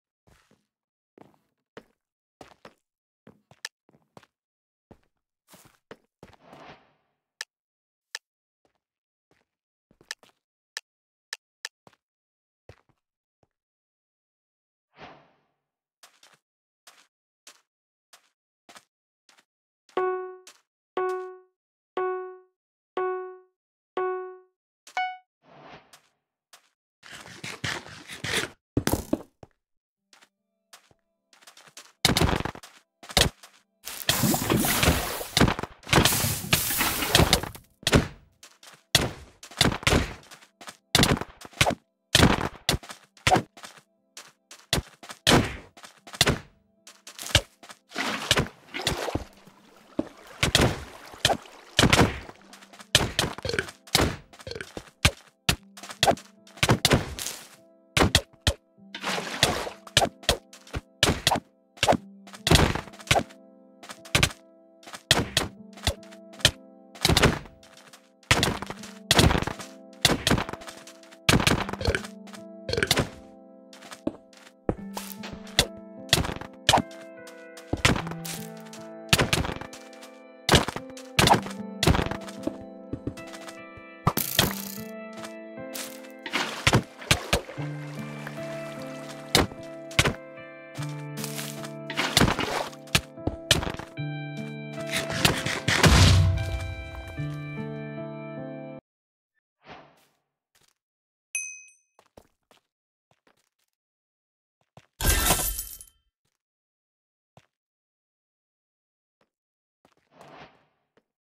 This guy trash talked me and thought he was good
So this guy beat me while having bad Internet connection so he takes no kb and wither sickles, one of the strongest legendaries. I had corrupted crossbow so I can only run and shoot crossbow. He then proceed to call me "noob", "c*nt" and many more words I won't repeat. He also mentioned he would destroy me in 1v1 so here is a video of me beating his ass in that 1v1. :) I recorded the whole thing so tell me if I should expose him :DD